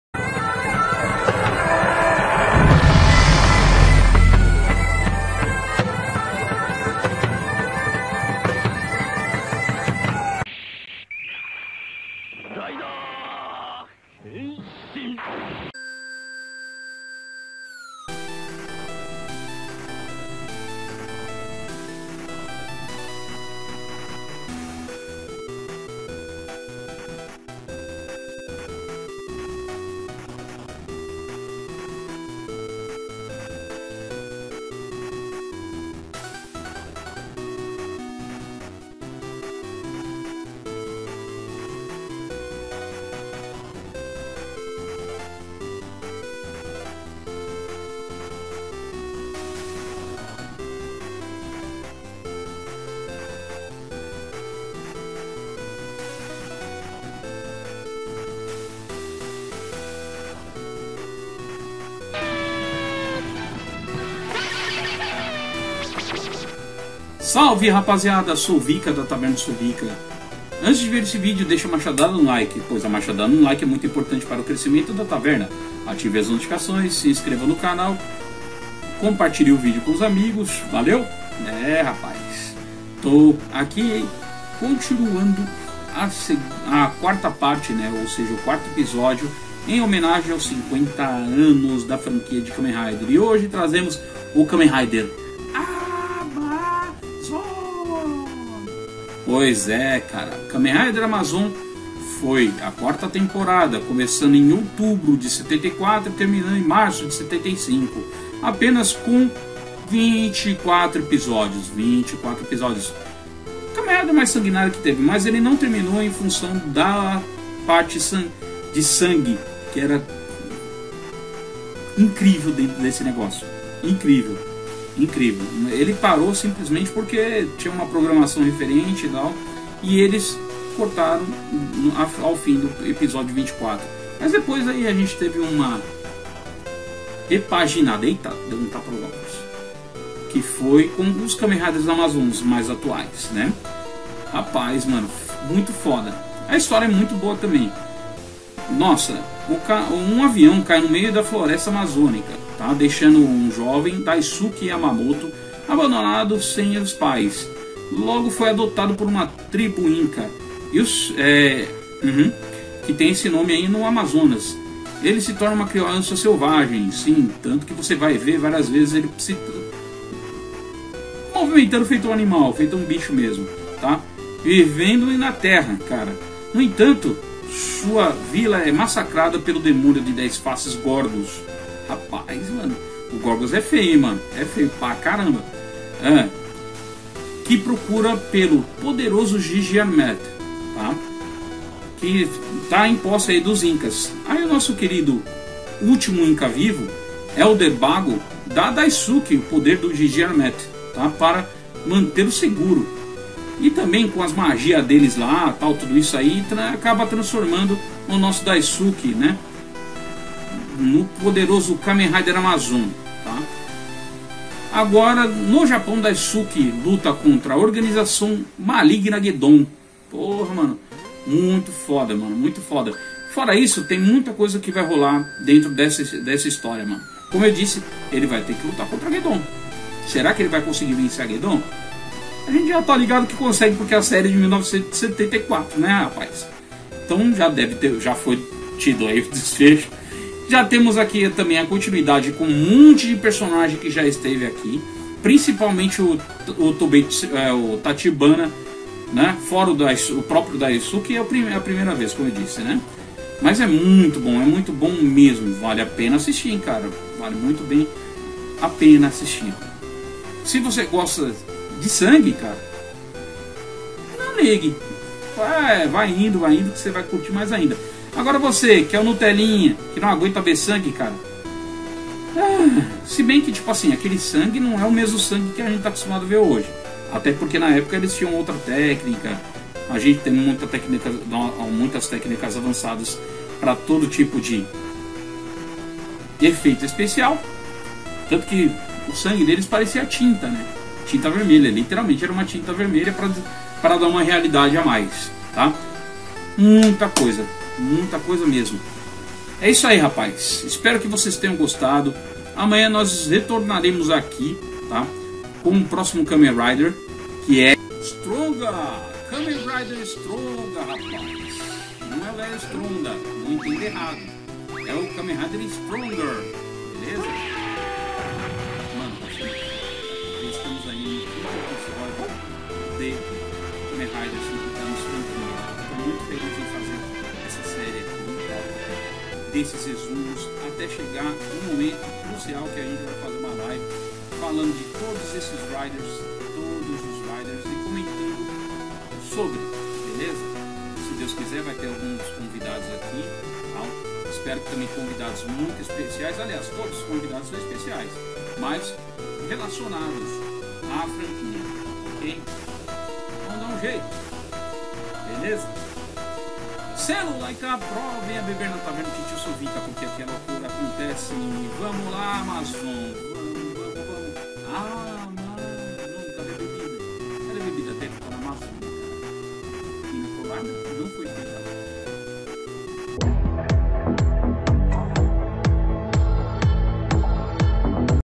50 anos da franquia kamen rider episódio 4 kamen rider amazon
FALA  RAPAZIADA HOJE É DIA DE FALAR DO KAMEN RIDER MAIS SAGRENTO DA ERA SHOWA O KAMEN RIDER AMAZON